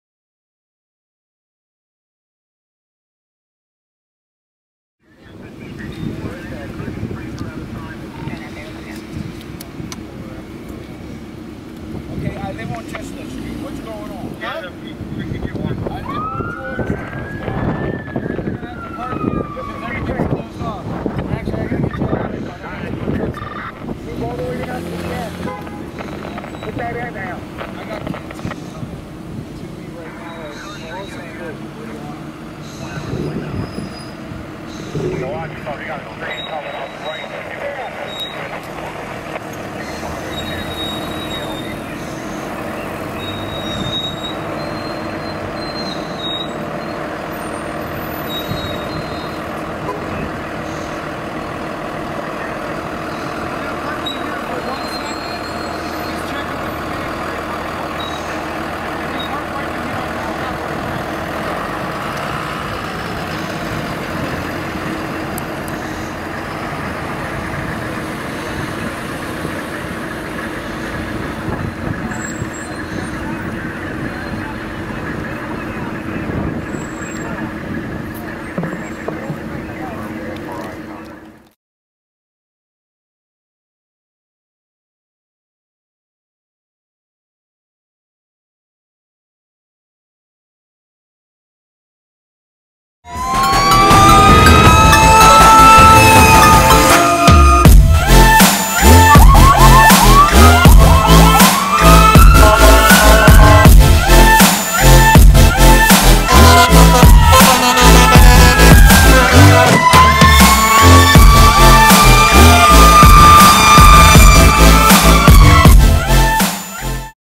North Penn Goodwill Service Canteen 41-8 & Shelter 41-7 Responding 11/2/20
North Penn Goodwill Service Canteen 41-8 & Shelter 41-7 Responding to a 3rd alarm working building fire in Norristown Borough, Montgomery County PA.